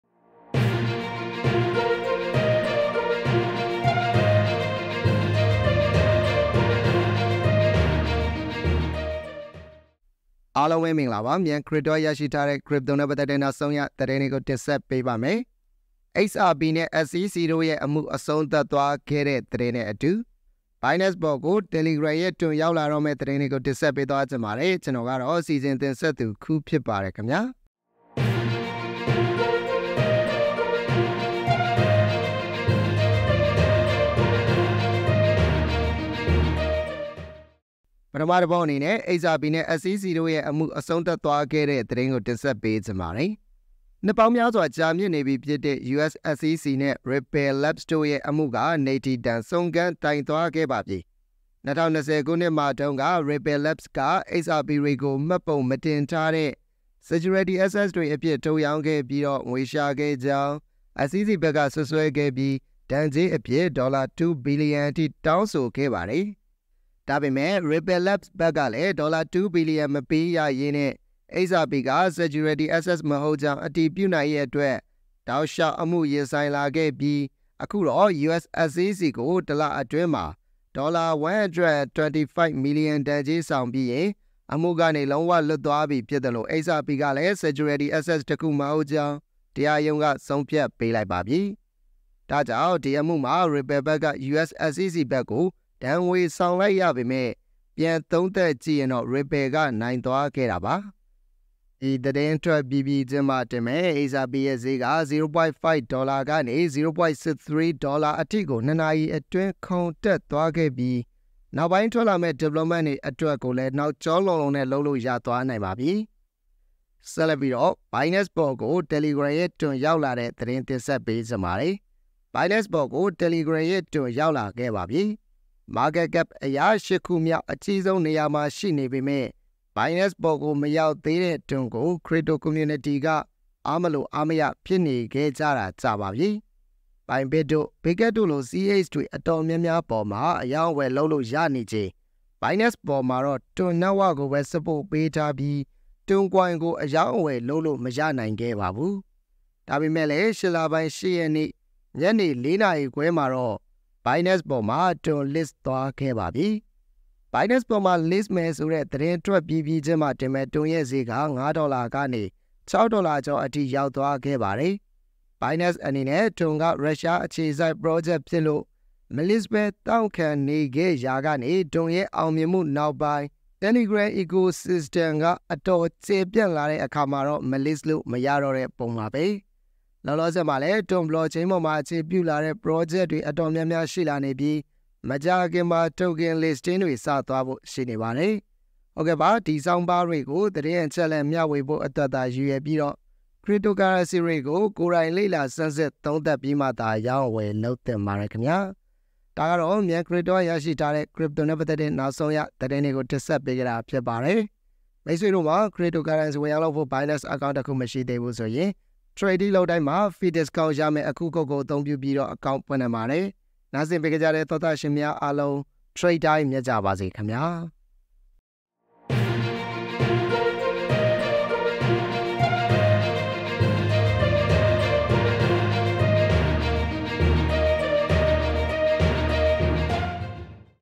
XRP နဲ့ SEC တို့ရဲ့ အမှုအဆုံးသတ်သွားခဲ့ပြီ | MCM Daily News (9th August 2024)
Crypto ကိုဘယ်ကစရမလဲ လေ့လာနေသူများအတွက် Study Room on Discord
Link to Myan Crypto Facebook